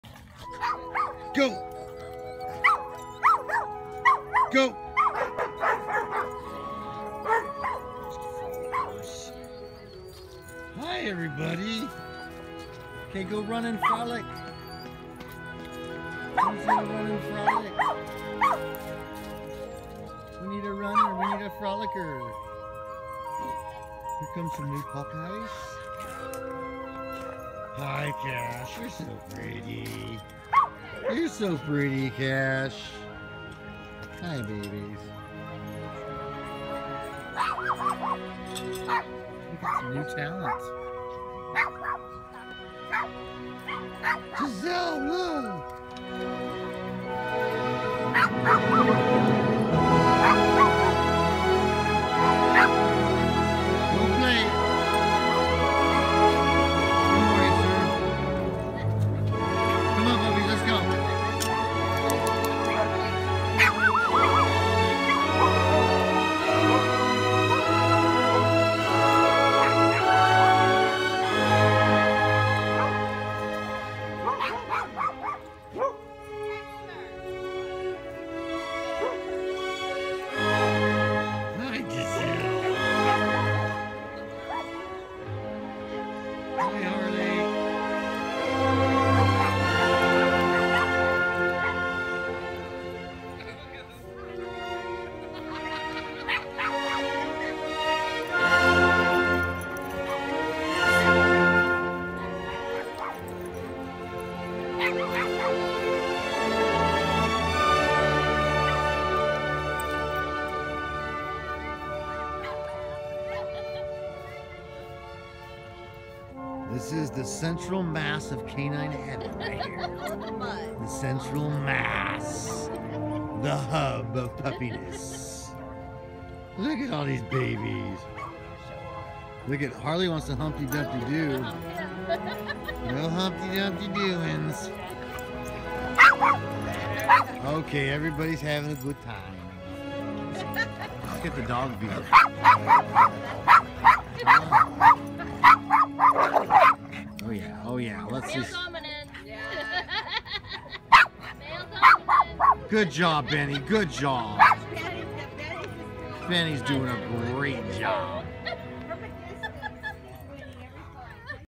K9 Frolic Fest #$barberDogpark
via YouTube Capture-- all the reg frolickers Harley, Cash, Gabbey, Eva, Lala, Jack etc etc